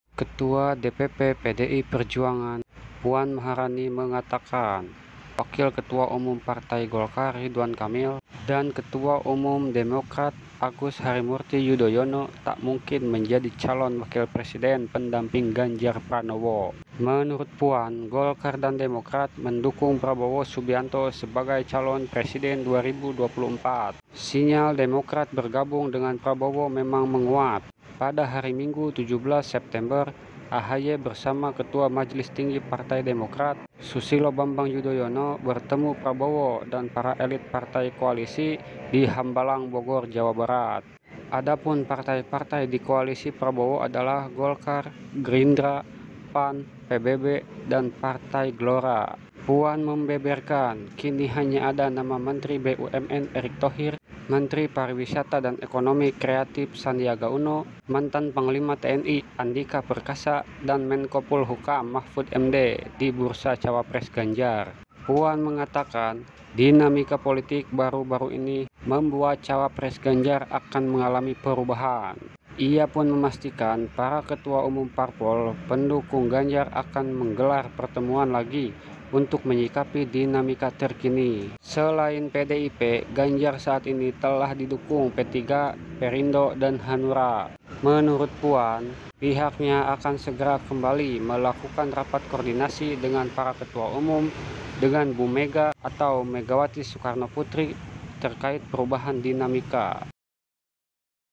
RIDWAN KAMIL DAN AHY DI CORET
Ketua DPP PDI Perjuangan (PDIP) Puan Maharani mengatakan Wakil Ketua Umum Partai Golkar Ridwan Kamil (RK) dan Ketua Umum Partai Demokrat Agus Harimurti Yudhoyono (AHY) tak mungkin menjadi calon wakil presiden pendamping Ganjar Pranowo.